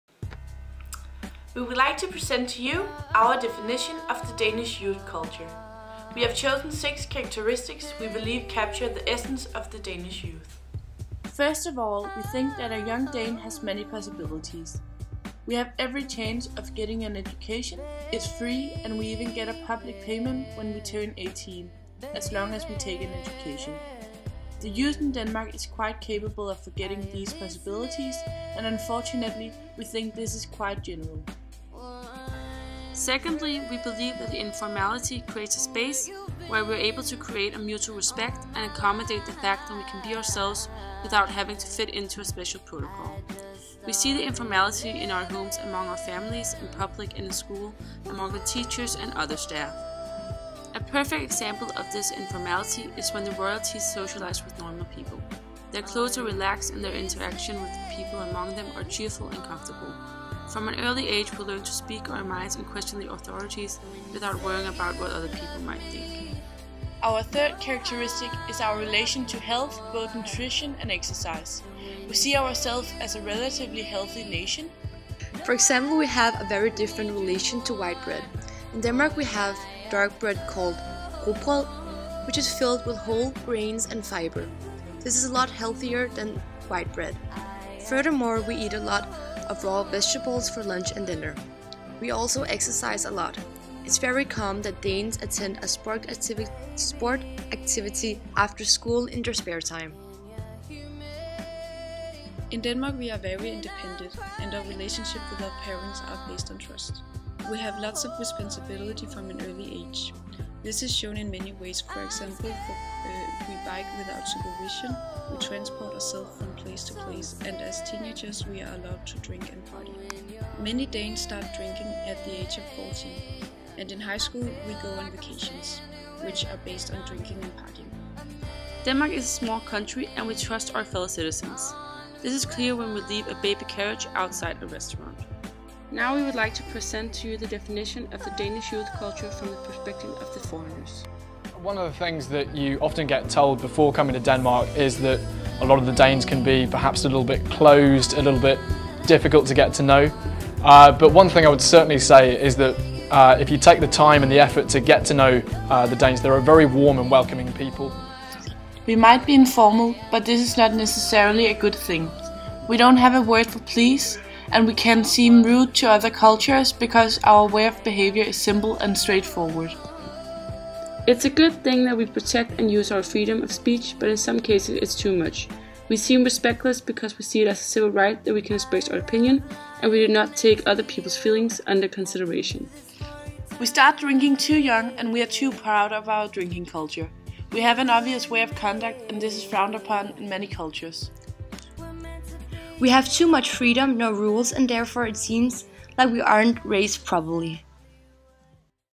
2.p laver film om kulturmødet
I uge 4 har 2.p på OG arbejdet med AT-emnet "Kulturmødet". Da de skal til Rom i uge 11, har fokus været på formidlingen af dansk kultur til italienske unge. De har, udover en fuld synopsis, arbejdet med at skulle skabe et innovativt produkt i form af enten en engelsksproget brochure eller en kort film på engelsk. Denne video er en af disse produkter fra forløbet, den er lavet af Clara, Maria, Madeline og Caroline.